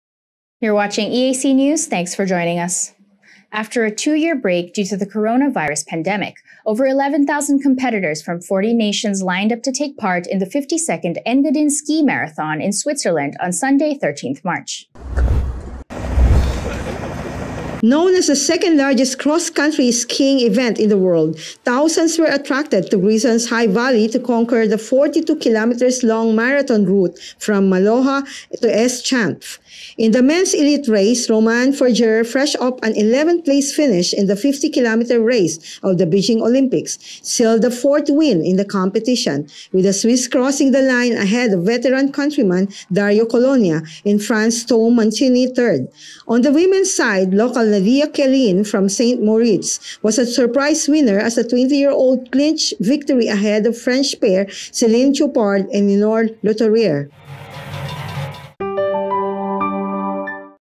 Thousands Take to The Snow as The Engadin Ski Marathon Returns
INTERNATIONAL: After a two-year break due to the coronavirus pandemic, over 11, 000 competitors from 40 nations lined up to take part in the 52nd Engadin Ski marathon in Switzerland on Sunday 13TH March.

Known as the second-largest cross-country skiing event in the world, thousands were attracted to Grisons high valley to conquer the 42-km long marathon route from Maloja to S-chanf.

In the men’s elite race, Roman Furger, fresh off an 11th-place finish in the 50km race at the Beijing Olympics, sealed a fourth win in the competition, with the Swiss crossing the line ahead of veteran countryman Dario Cologna, and France’s Tom Mancini third.

On the women’s side, local Nadja Kalin from St. Moritz was a surprise winner as the 20-year-old clinched victory ahead of French pair Celine Chopard and Enora Latuilliere.